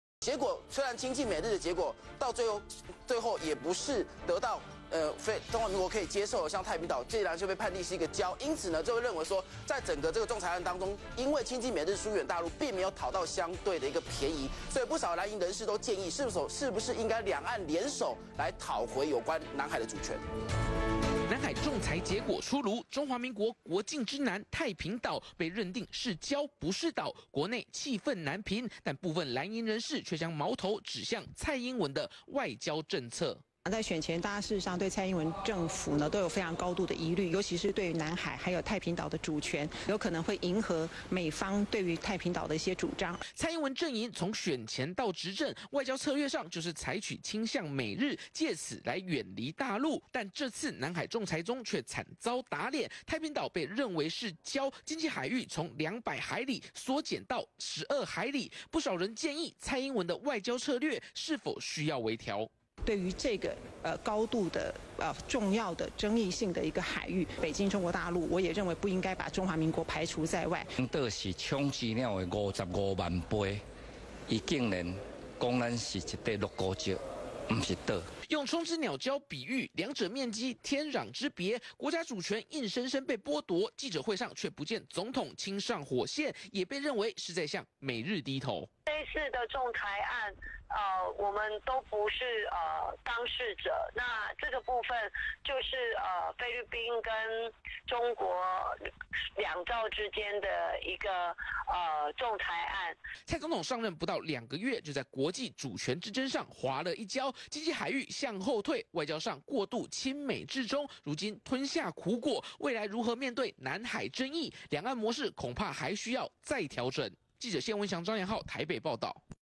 蔡「親美制中」討嘸便宜 仲裁仍輸菲律賓
中天新聞   2016年07月13日 11:55 謝文祥、張良晧

南海仲裁結果出爐，中華民國國境之南太平島，被認定為是礁不是島，國內氣憤難平，但藍營人士卻將矛頭，指向蔡英文的外交政策。

蔡英文陣營從選前到執政，在外交策略上，就是採取傾向美日，藉此來遠離大陸，但在這次南海仲裁中，卻慘遭打臉，太平島被認定為礁，經濟海域從200海浬，縮到只剩12海浬，不少人士建議，小英的外交政策是否需要微調？

用沖之鳥礁作比喻，兩者面積天壤之別，國家主權硬生生被剝奪，記者會卻不見總統親上火線，也被認為是在向美日低頭。

蔡總統上任不到2個月，就在國際主權之爭上滑了一跤，經濟海域向後退，外交上過度親美制中，如今吞下苦頭，未來如何面對南海爭議，兩岸模式恐怕還需再調整。